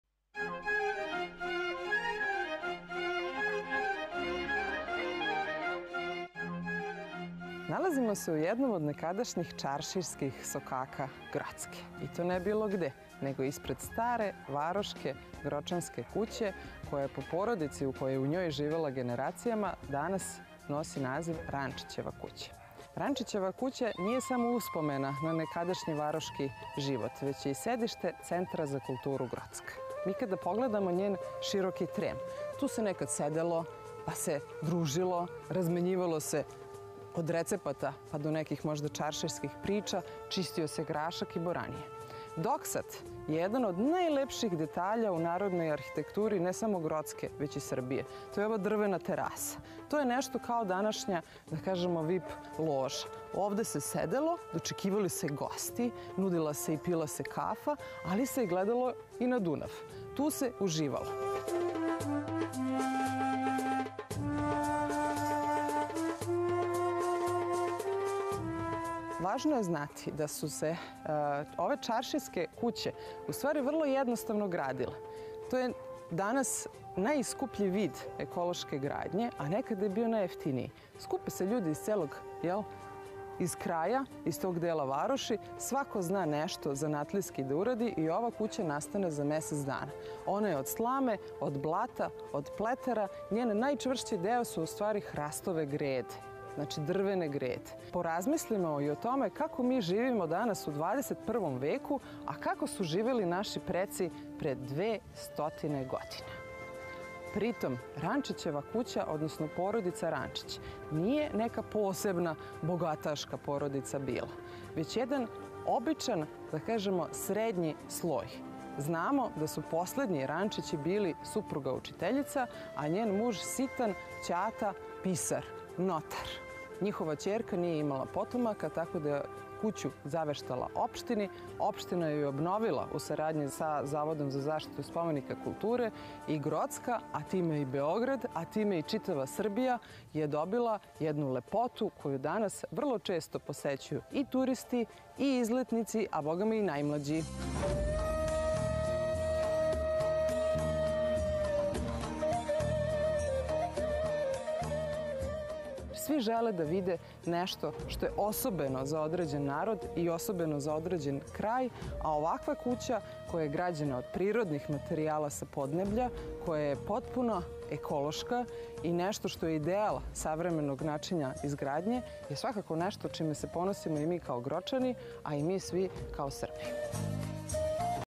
TV Happy: Зорица Атић о Ранчићевој кући у Гроцкој
У прилогу за TV Happy, директорка Центра за културу Гроцка Зорица Атић говори о Ранчићевој кући у Гроцкој. Стара варошка кућа бележи више од два века трајања и око четири деценије културне намене, а сваком посетиоцу дарује незаборавну атмосферу свог ванвременског склада и спокоја. Име је добила по породици Ранчић којој је готово два века припадала. Грађена од материјала са поднебља без иједног ексера, широких стреха, са дрвеним доксатом и кровом од ћерамиде, драгоцен је примерак очуваног народног неимарства. Кућа је заштићена као културно добро од великог значаја 1966. године. Године 1982. године у њој је отворен „Завичајни музеј Гроцке“, са драгоценом археолошком и личном заоставштином др Александра Ђ. Костића. Ранчићева кућа је од 2006. године „домаћин“ установи Центар за културу општине Гроцка, a mузејска поставка се данас налази у галерији библиотеке „Илија Гарашанин“ у Гроцкој.